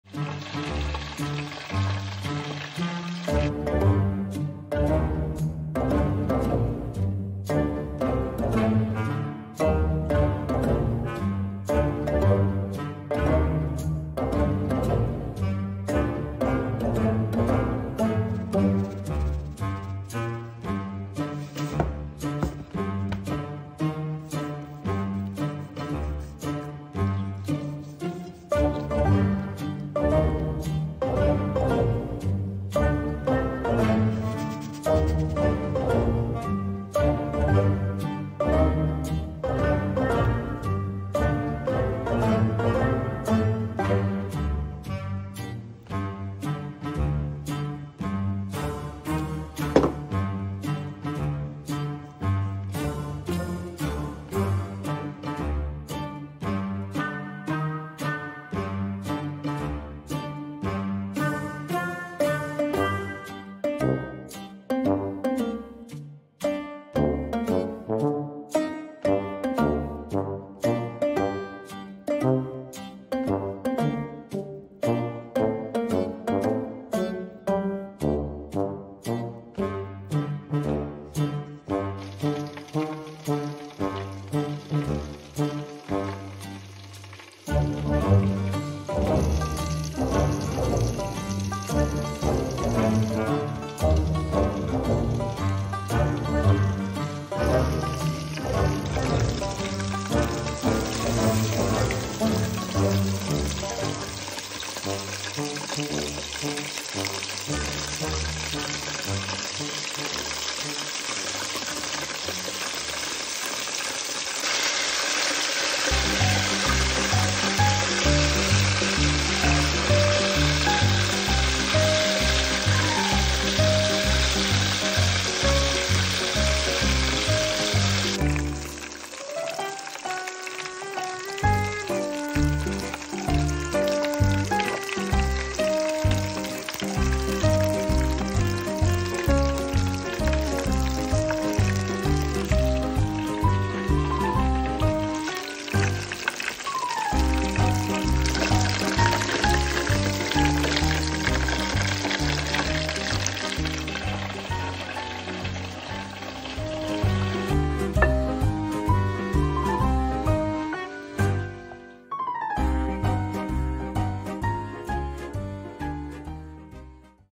Как вкусно пожарить рыбу с хрустящей корочкой
Секрет вкусно пожаренной рыбы в хрустящей корочки. Как этого добиться? Можно узнать из этого видео